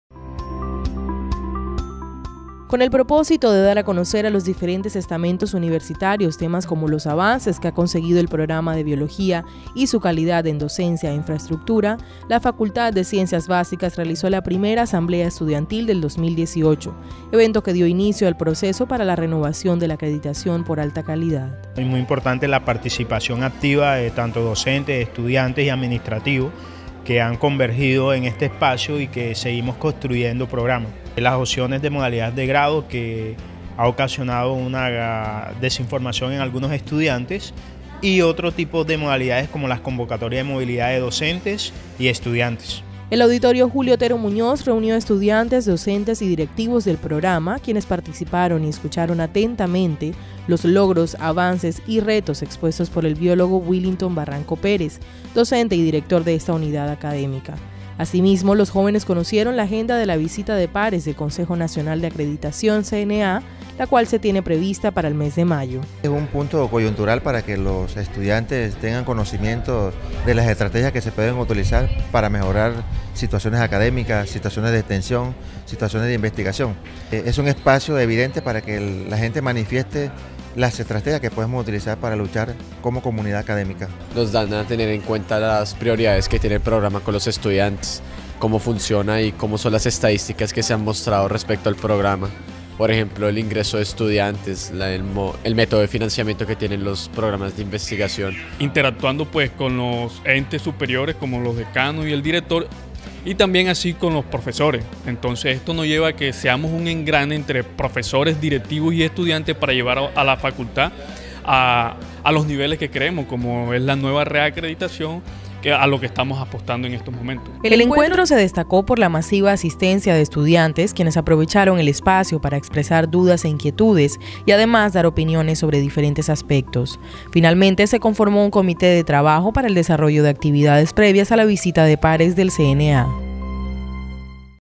Boletín Audiovisual 064 Programa de Biología UNIMAGDALENA inició renovación de la acreditación
A través de una asamblea estudiantil,  la Facultad de Ciencias Básicas de la Alma Mater, socializó a la comunidad universitaria los procesos de autoevaluación y planes de mejoramiento que prevé la unidad académica.